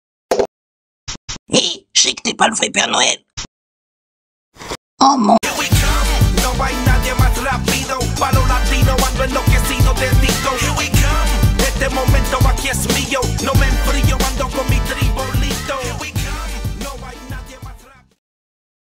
relax max n° 196 : LE VRAI PERE NOEL ?
vous qui avez été sage durant cette année, en plus d'avoir des cadeaux, aurait on trouvé le vrai père noël ? xD

Joyeux Noël et bonnes fêtes !